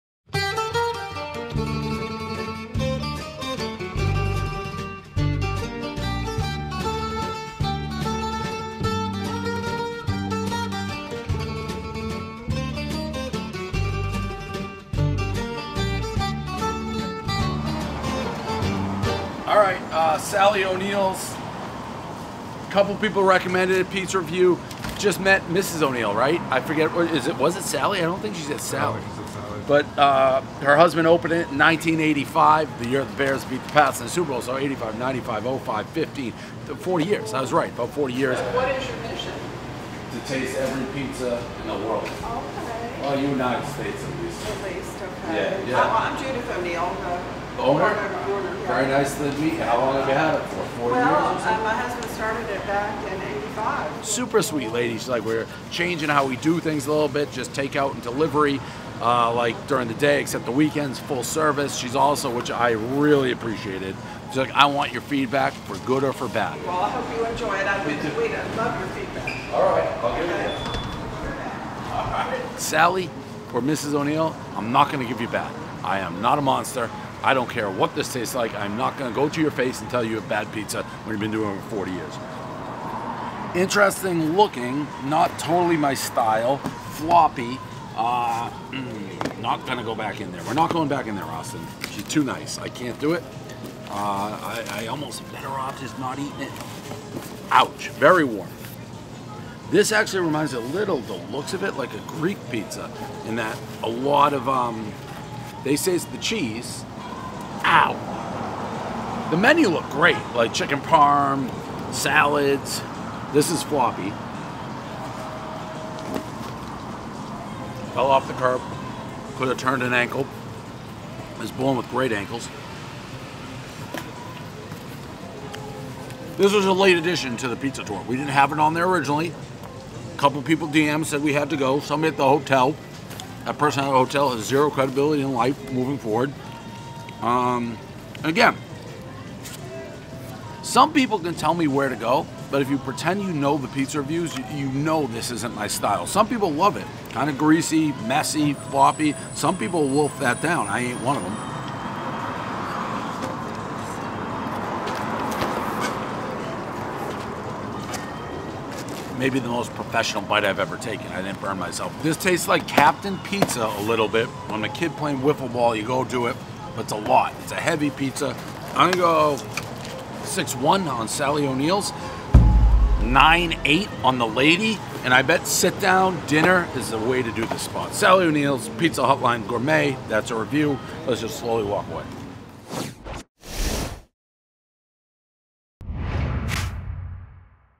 Barstool Pizza Review - Sally O' Neal's Pizza Hotline (Tampa, FL)
Dave has a late addition to the pizza trip with Sally O'Neals, with a very sweet owner who wants feedback "for good or for bad".